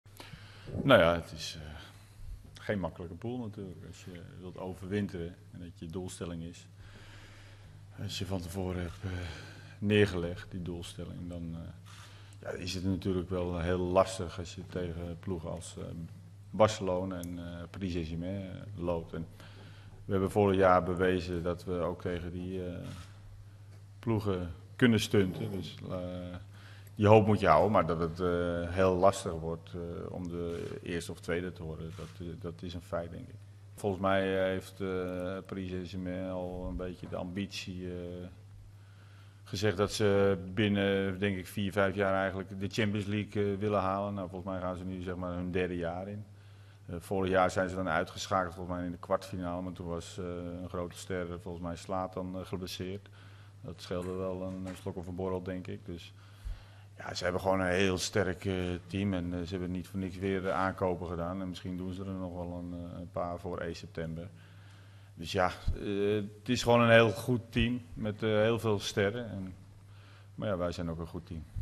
Frank de Boer: 'Het wordt lastig'
Frank de Boer na de loting voor de Champions League: 'Het wordt lastig'. Lees verder